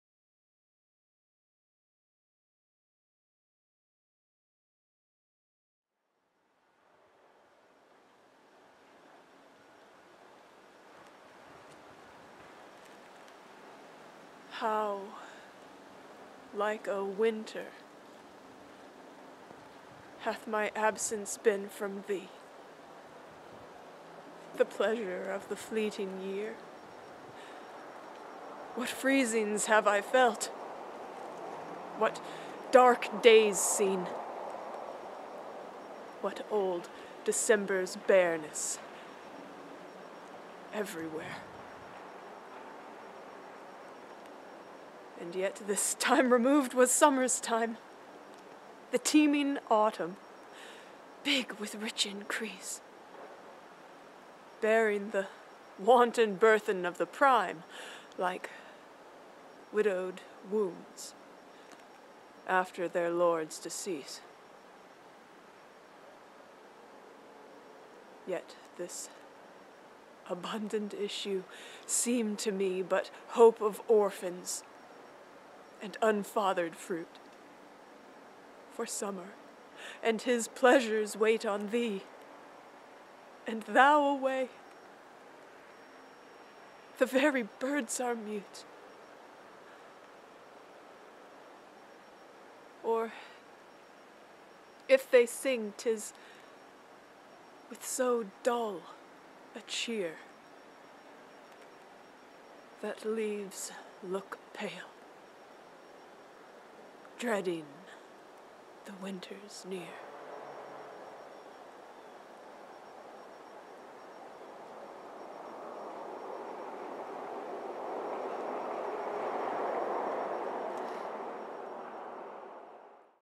William Shakespeare's Sonnet 97
Christine Williamson of Moonsong Studios performs Shakespeare's Sonnet 97.